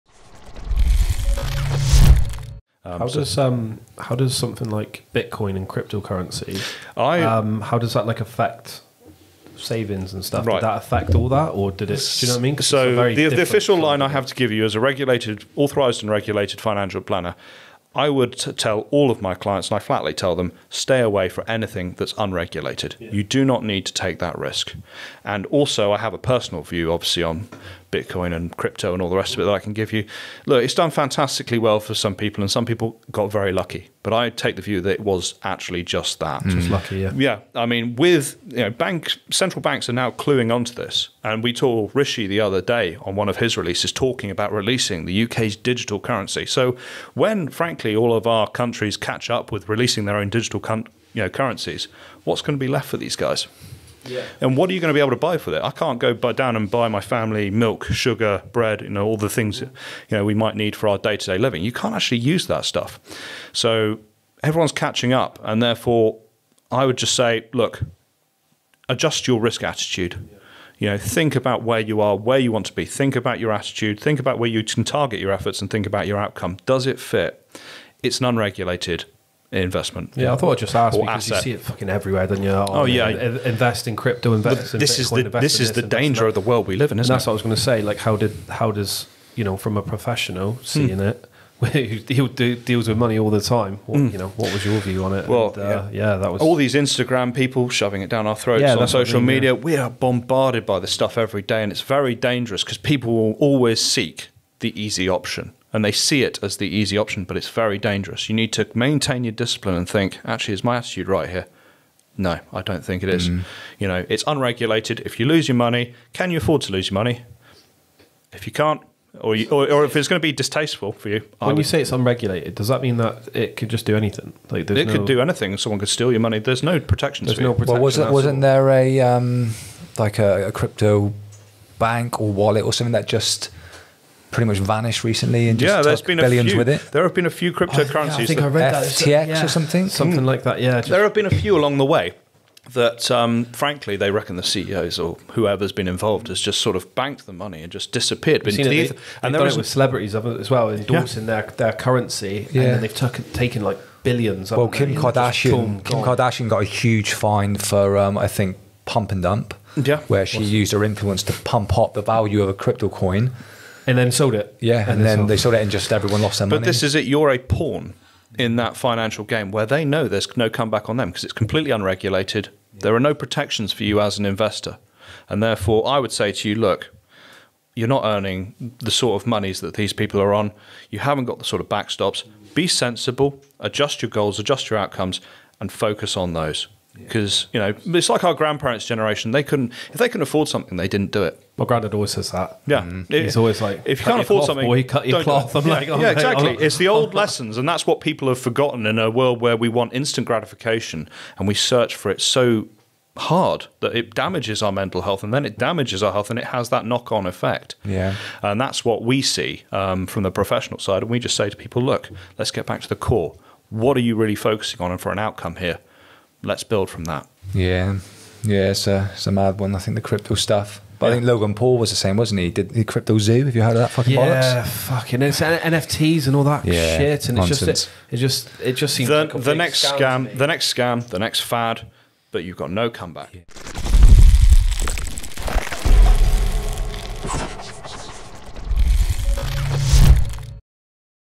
Regulated Advice on Investing in Cryptol | The Everyday Perspective Podcast Clips
Regulated financial advisor, stoke broker and wealth manager advises on if Crypto is a sound investment